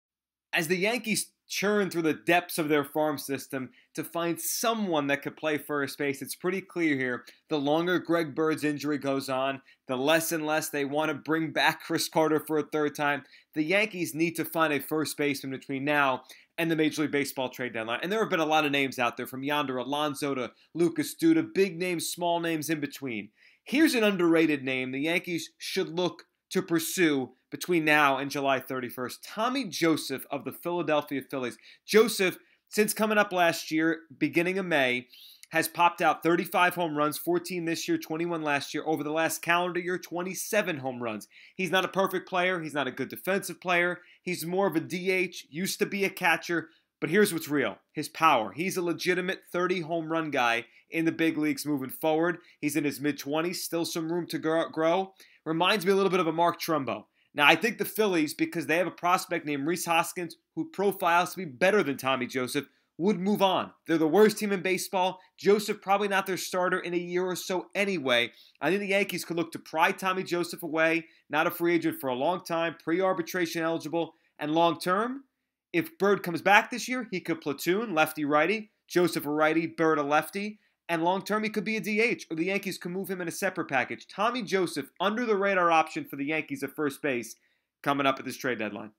Under-the-radar first base trade option for Yankees' Brian Cashman
The Yankees need a first baseman. Phillies slugger Tommy Joseph could be a good fit as GM Brian Cashman ponders a move before the non-waiver trade deadline on Monday, July 31, 2017 (7/31/17) with the future of Greg Bird in doubt. 7/6/17 (Joe Giglio | NJ Advance Media for NJ.com)